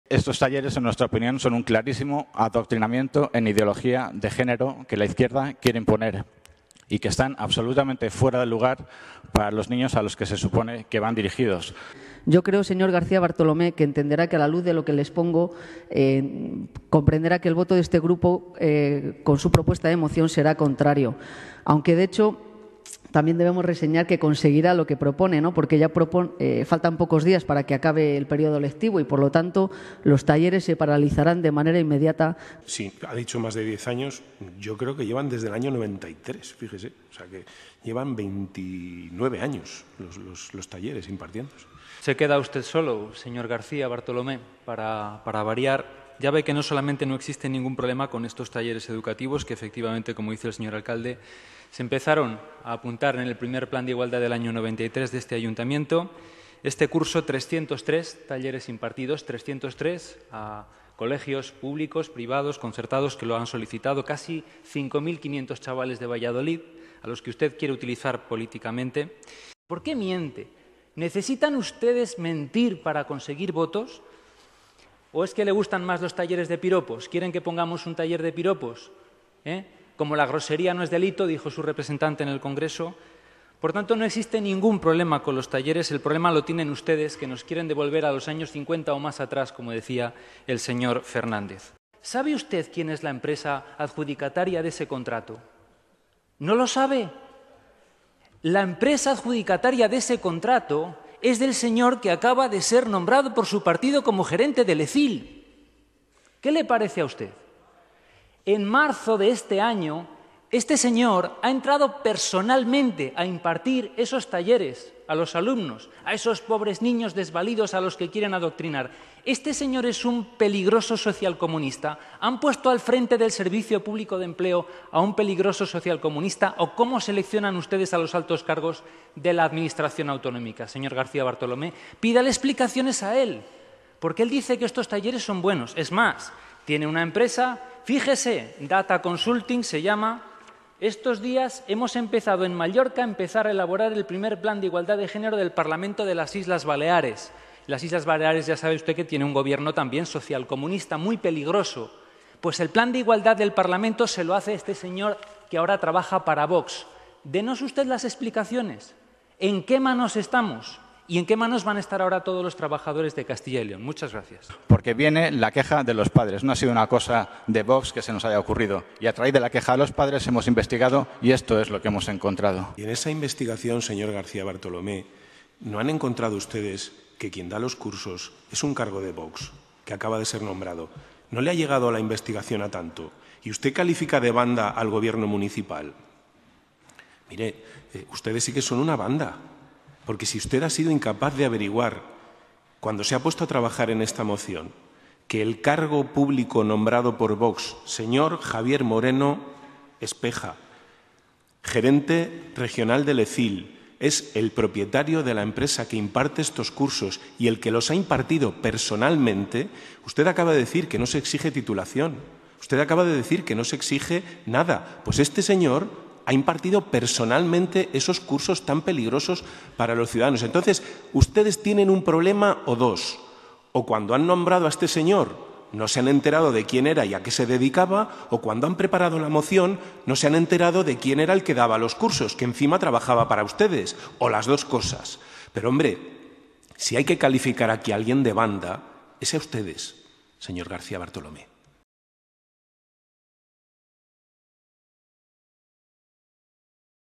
Dimite alto cargo de Castilla y León que nombró Vox, por dar cursos igualdad
El recién nombrado por Vox gerente del Servicio Público de Empleo de Castilla y León (Ecyl), Javier Moreno Espeja, ha dimitido este lunes tras conocerse, precisamente por las críticas de Vox, que su empresa Data Consulting ha sido la encargada de impartir cursos de igualdad y educación sexual en más de 300 colegios de Valladolid y que él mismo había ejercido de monitor en alguna de las charlas.

Esta renuncia se ha producido solo unas horas después de que, en el Pleno del Ayuntamiento de Valladolid, el único concejal de Vox, Javier García Bartolomé, defendiera una moción en la que pedía paralizar los cursos al considerar que eran "charlas ideológicas al más estilo podemita", que tenían por objetivo "adoctrinar" a los escolares.

Vídeo: EFE